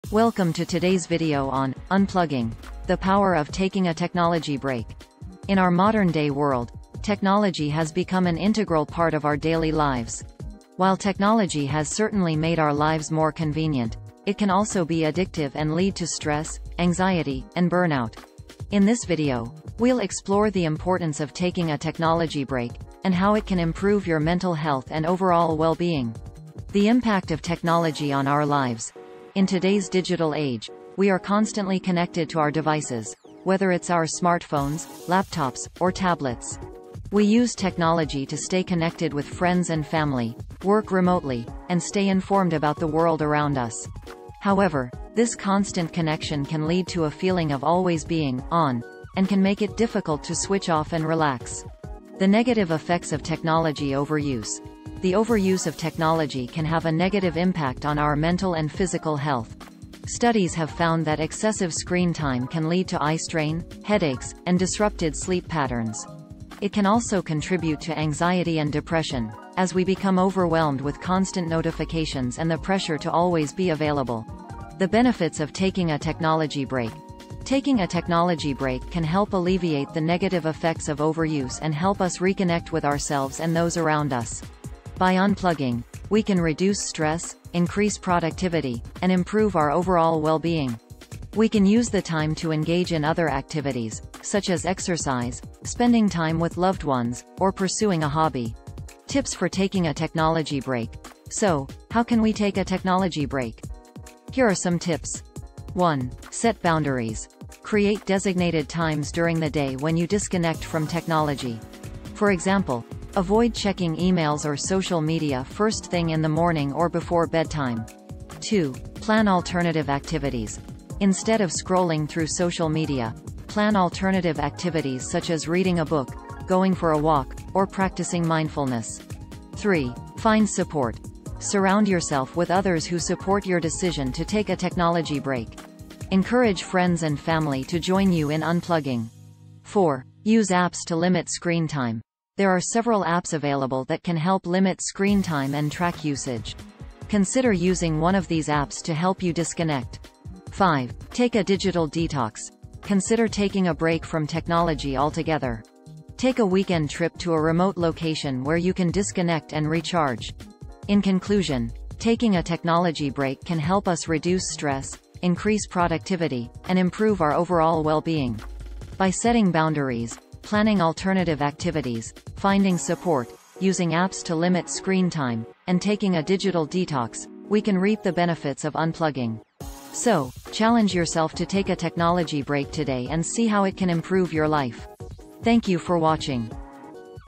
Unplugging: The Power of Taking a Technology Break
In this video, we explore the impact of technology on our lives and the negative effects of overuse. We discuss the benefits of taking a technology break and provide tips on how to disconnect from technology, such as setting boundaries, planning alternative activities, finding support, using apps to limit screen time, and taking a digital detox. By unplugging, we can reduce stress, increase productivity, and improve our overall well-being. Join us as we discover the power of taking a technology break and how it can transform our lives.